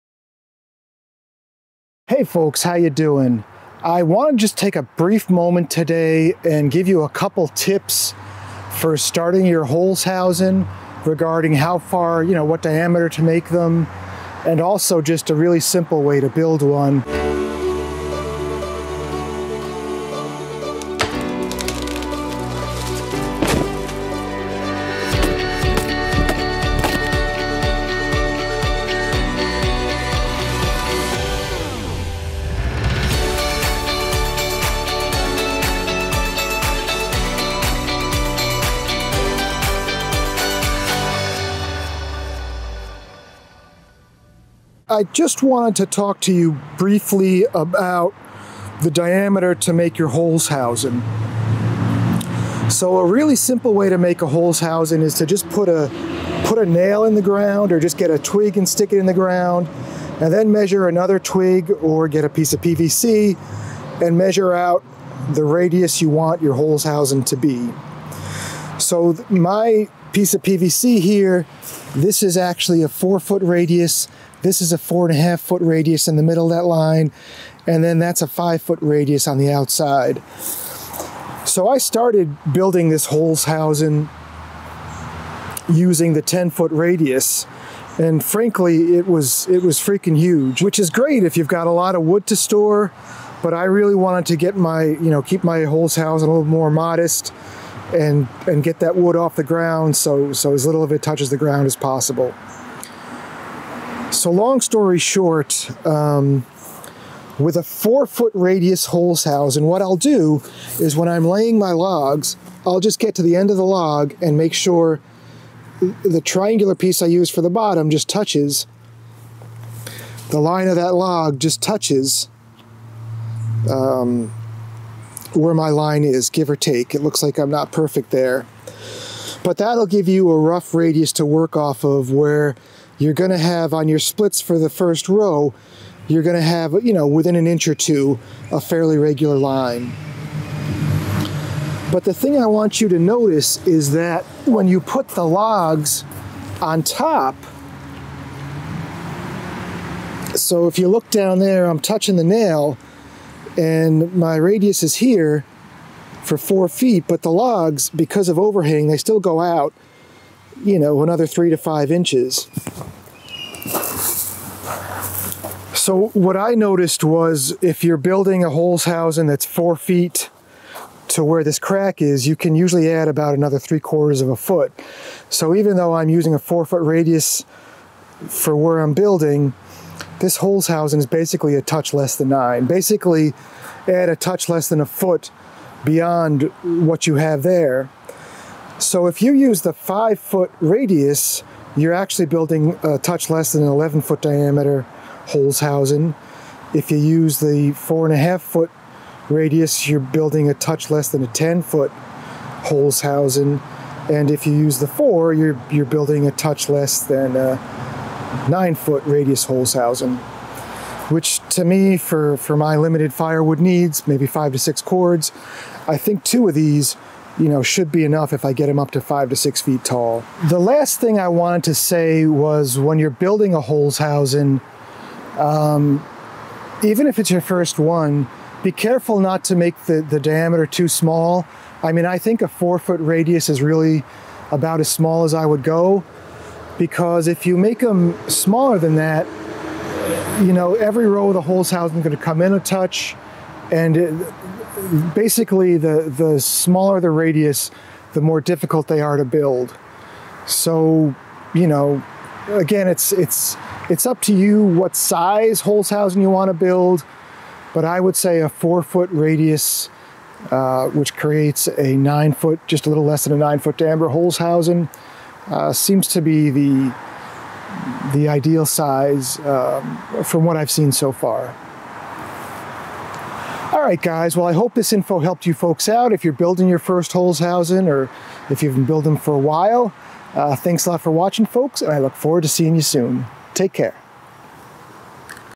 SECRET TIP to BUILDING a HOLZHAUSEN FIREWOOD PILE
If you are building a Holzhasuen Firewood Pile there is one secret trick to get your Firewood stack started off right.  In today's video, John shows you the secret to building a Holzhausen Firewood Pile.

RECOMMENDED TOOLS!!!*